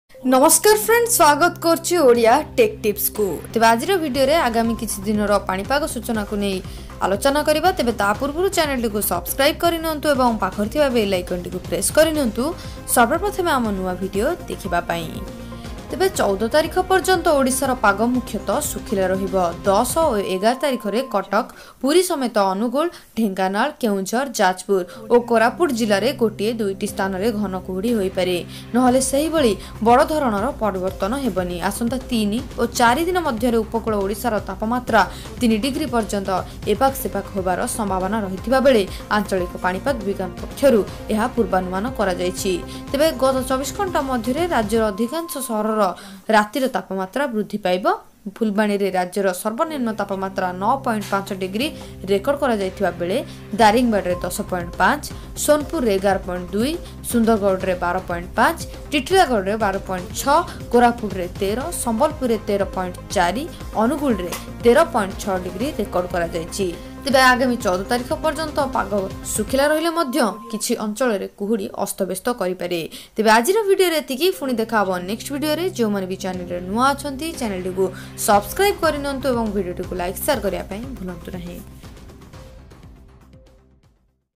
ମାଡି ଆସୁଛି ଭୀଷଣ ବର୍ଷା, ଆଗାମୀ ୨୪ ଘଣ୍ଟା ଭିତରେ ଏହି ସବୁ ଜିଲ୍ଲାରେ ହେବ ପ୍ରବଳ ବର୍ଷା ପବନ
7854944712 Rain forecast in next 24 hours in Odisha